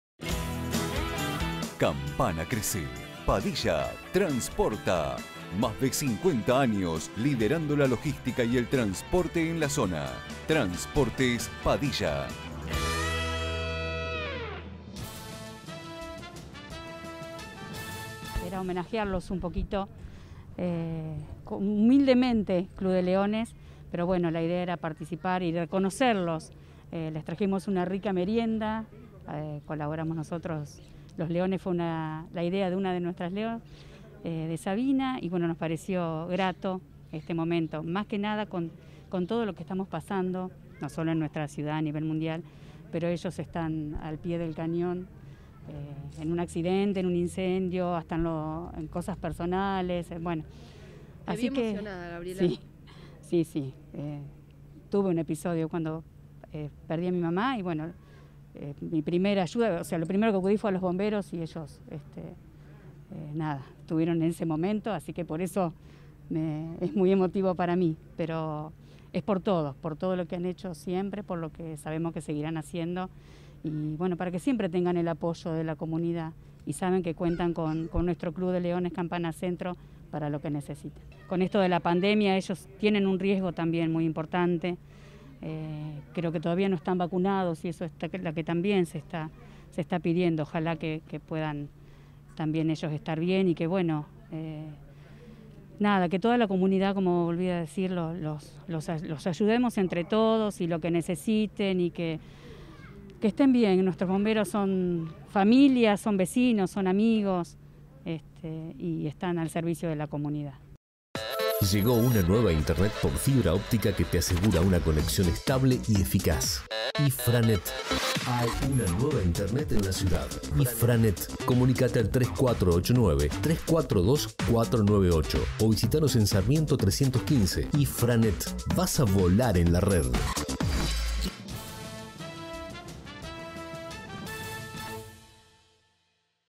EL CLUB DE LEONES HOMENAJEÓ A LOS BOMBEROS EN SU DÍA
En el día de ayer donde se conmemoró el Día del Bombero Voluntario, el Club de Leones de Campana se hizo presente en los homenajes para obsequiarle una merienda al cuartel como forma de agradecimiento a su labor y entrega.